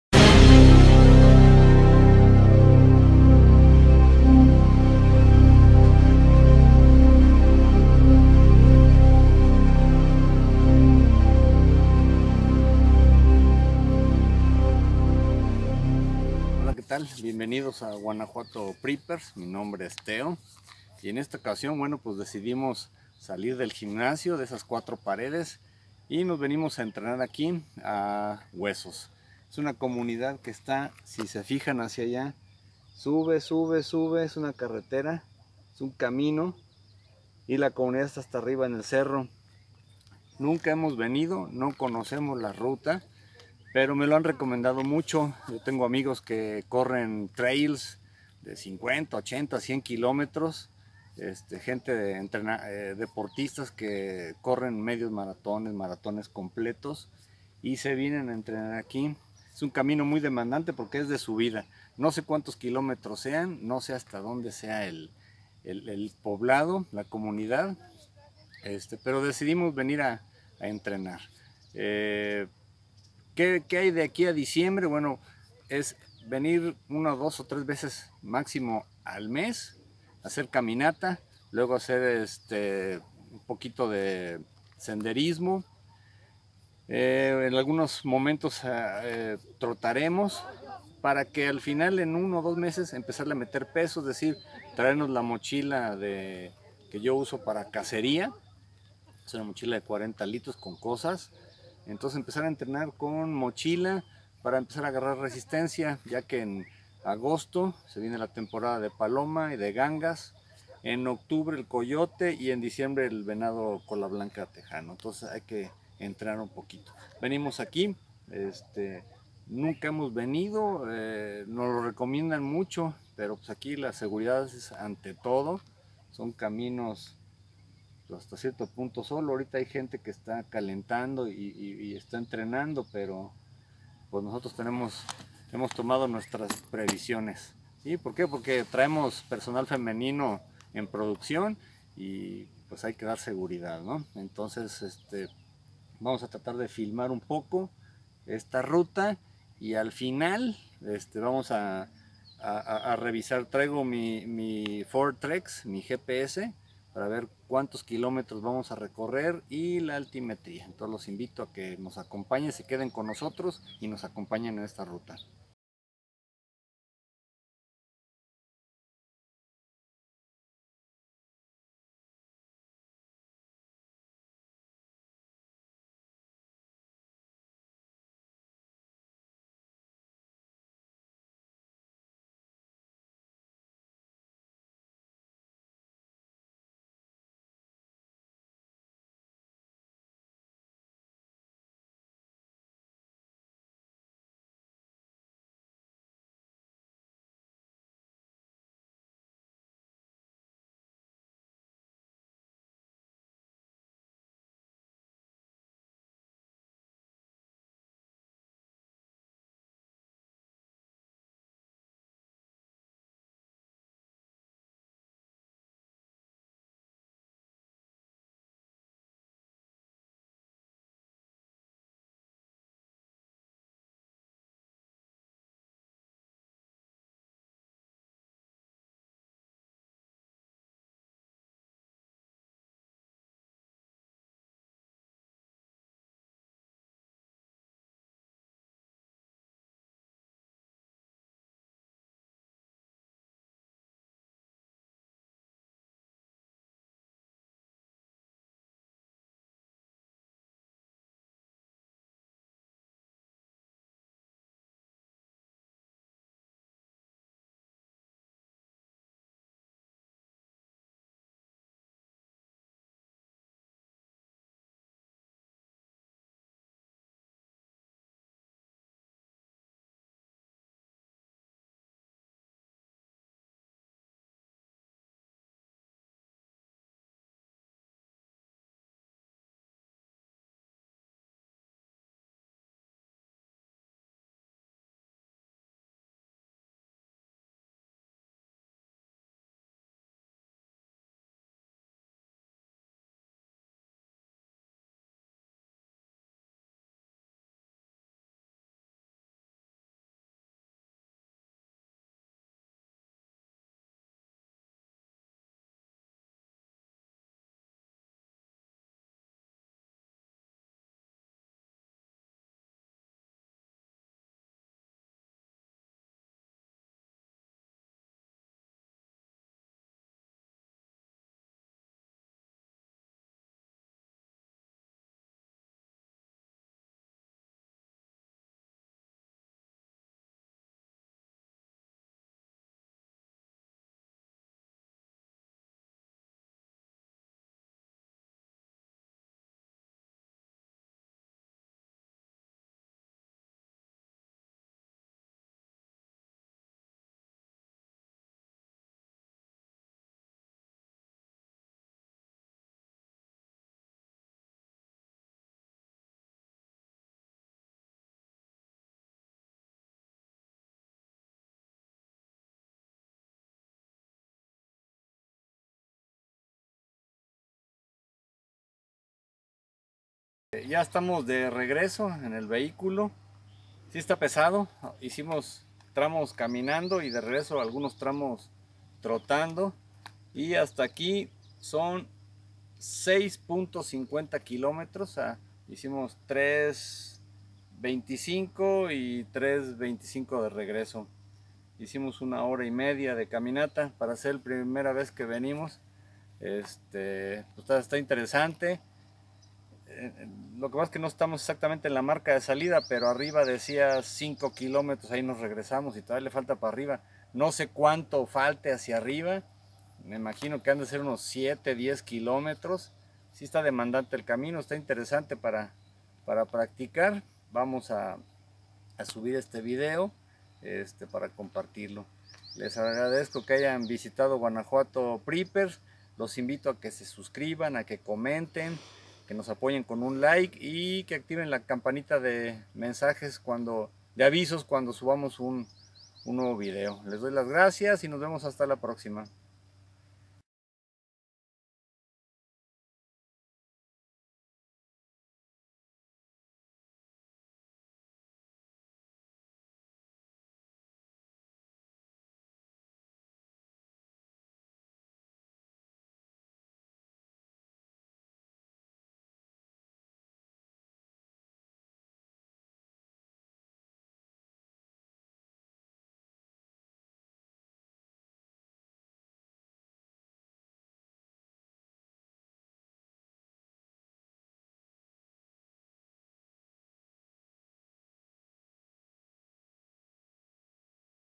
Caminata hacia la Comunidad de Huesos
Caminata para entrenamiento en la comunidad de huesos, recorrido de 6.5 kmts.

RUNNING-DELTA SPIRIT